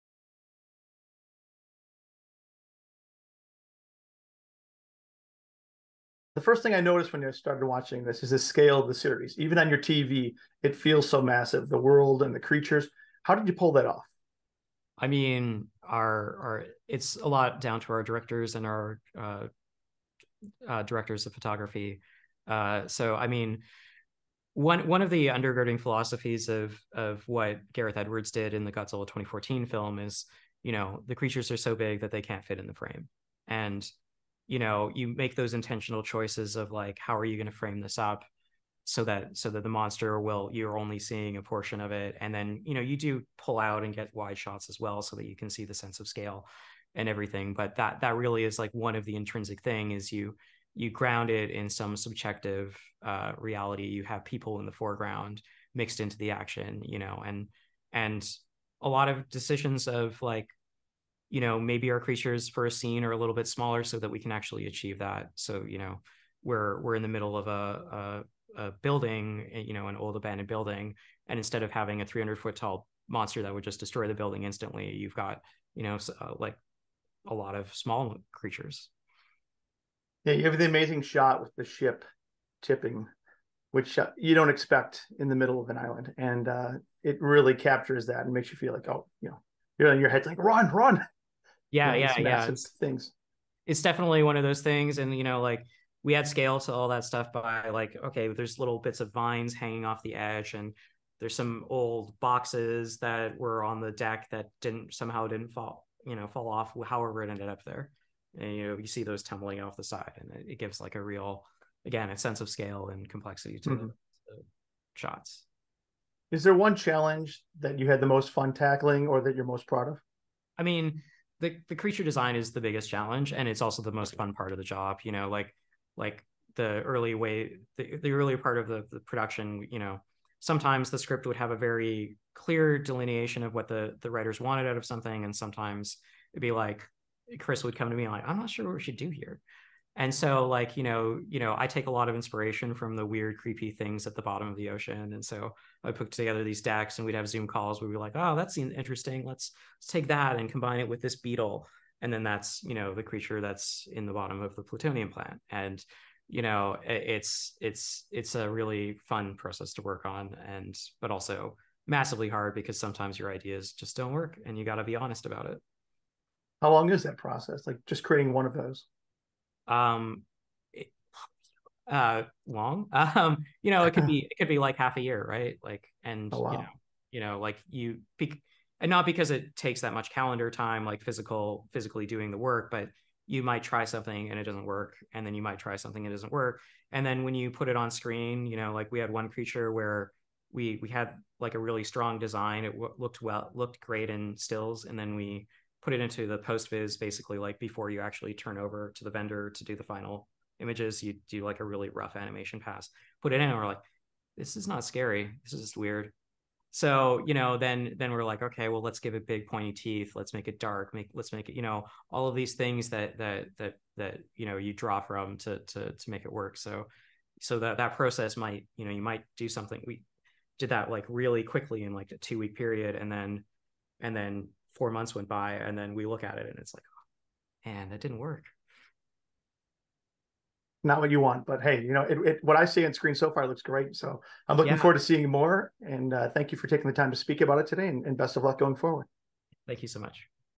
Monarch: Legacy of Monsters VFX Supervisor, Sean Konrad, On The Massive Scale of Godzilla-verse
Monarch: Legacy of Monsters VFX Supervisor, Sean Konrad, speaks with Awards Radar's Steven Prusakowski about how they make the audience feel the massive scale of the Godzilla-verse